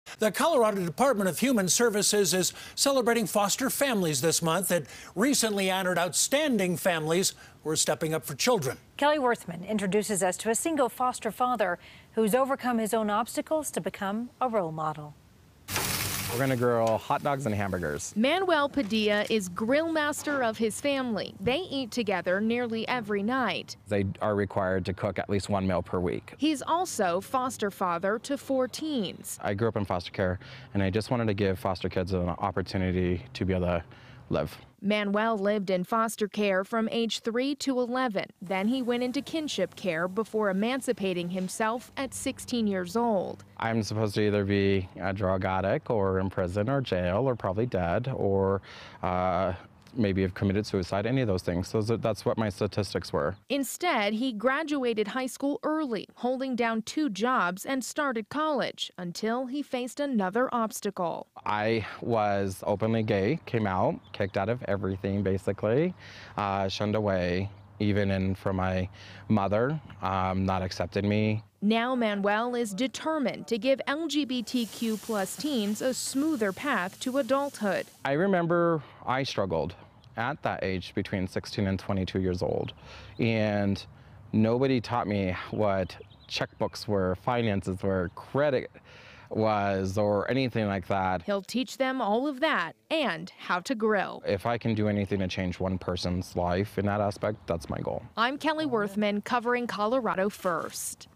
Foster Parents Stepping Up For Colorado Children Needing Stability
The Colorado Department of Human Services is celebrating foster families this month. On Saturday, May 8th, in a virtual event, the agency honored five foster families from across the state. One of the honorees was Manuel Padilla, who himself has overcome some pretty difficult obstacles to become a foster parent.